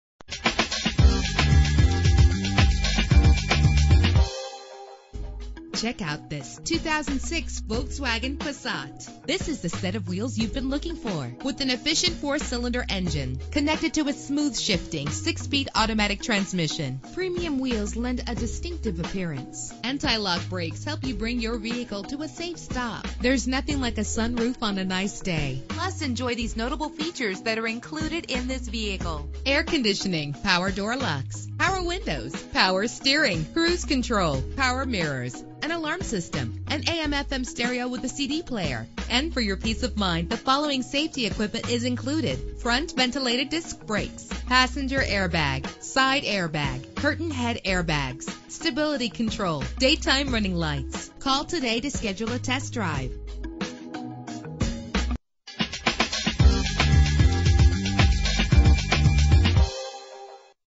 2006 Volkswagen Passat - Auburn WA
Year: 2006
Make: Volkswagen
Model: Passat
Trim: 2.0T
Engine: 2.0 liter inline 4 cylinder 16 valve
Transmission: 6-Speed Automatic
Color: Candy White
Mileage: 61231
Address:
4852 Auburn Way North